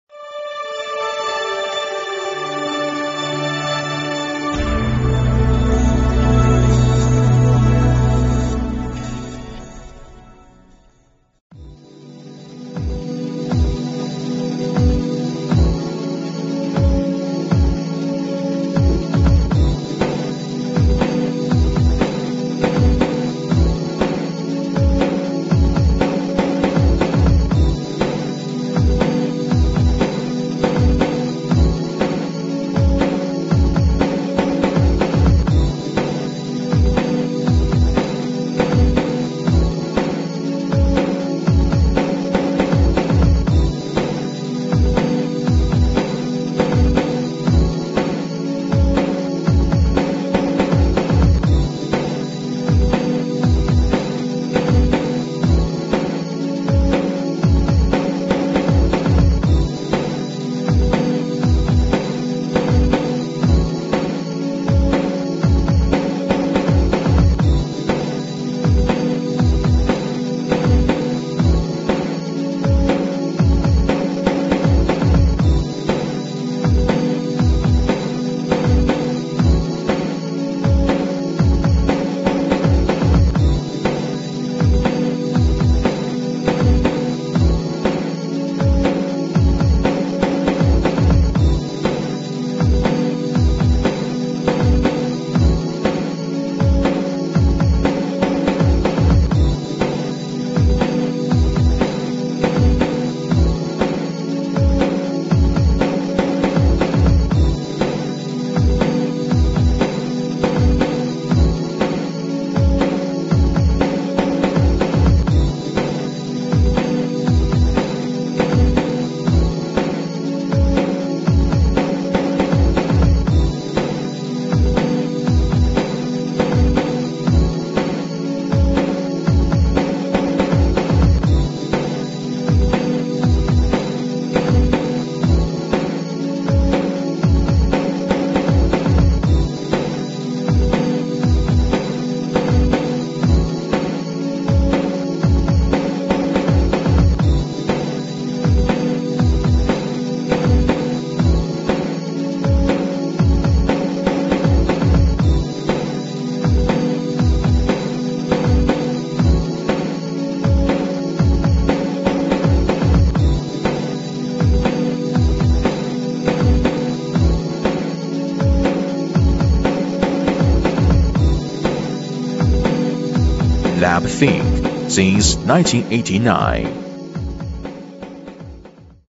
C620H Friction / Peel Tester
C620H Friction / Peel Tester can be used to test coefficient of static and kinetic friction of plastic films, sheets, foils, paper, cardboard, woven bags, fabric (fabric style test), metal-plastic composite strip/belt for communication cable, convey belt and textile, as well as peel strength test of adhesive laminated products, medical adhesive bandage, release paper and protection films.